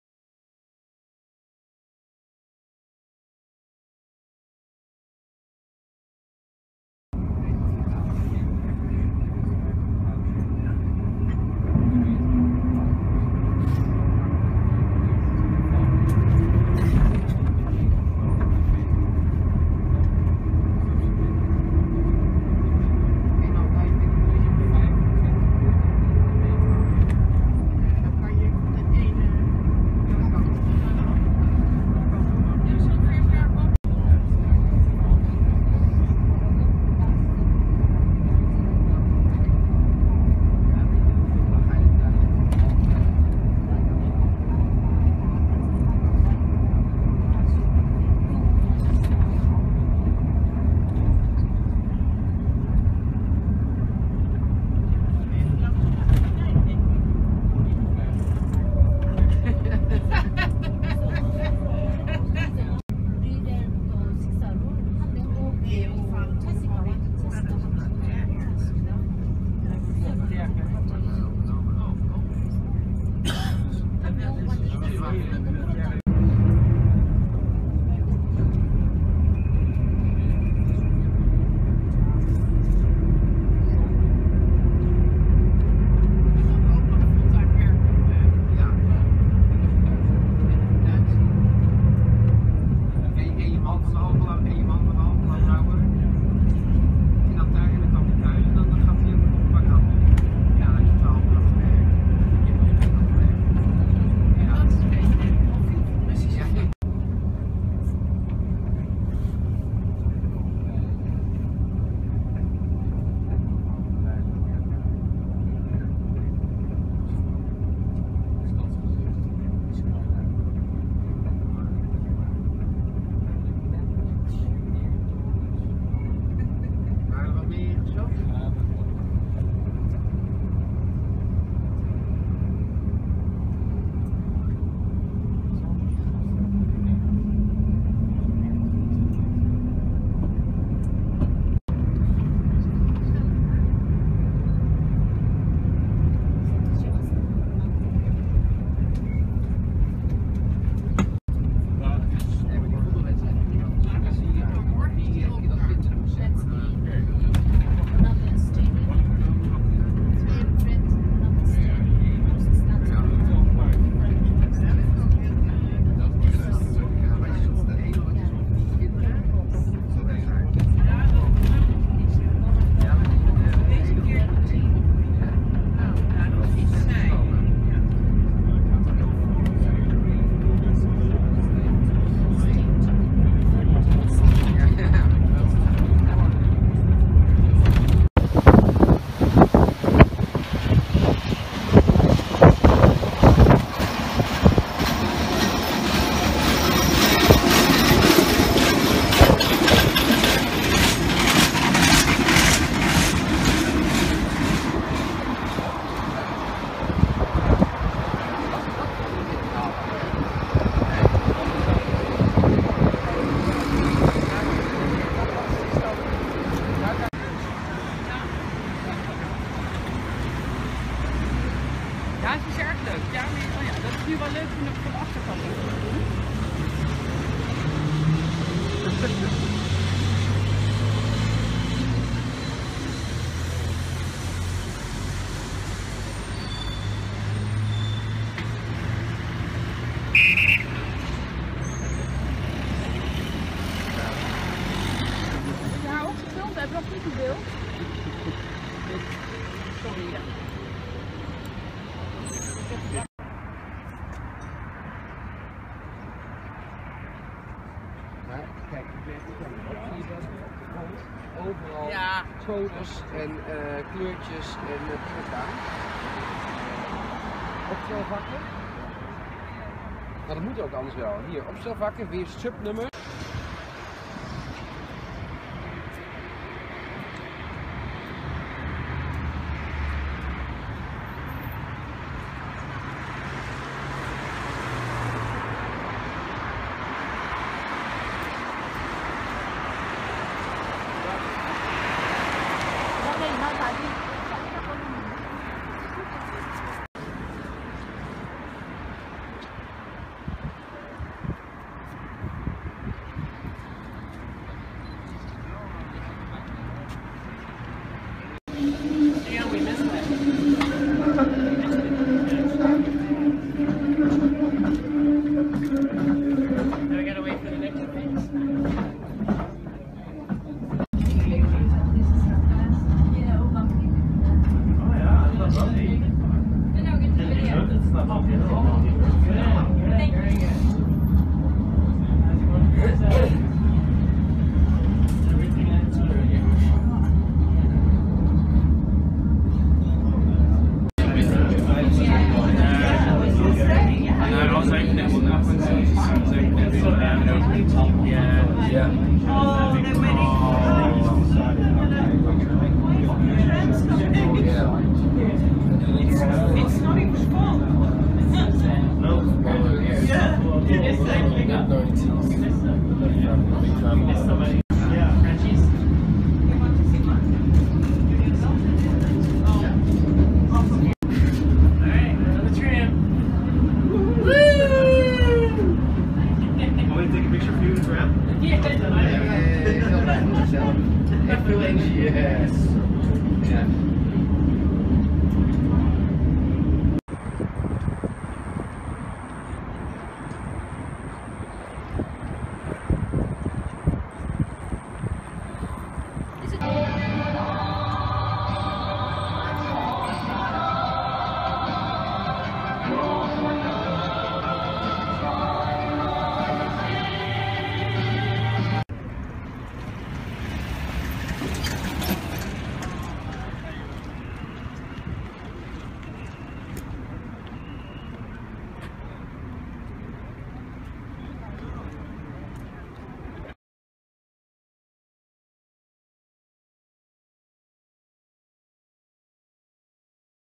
Driving Around Pyongyang (North Korea)
Driving around the streets of Pyongyang in North Korea.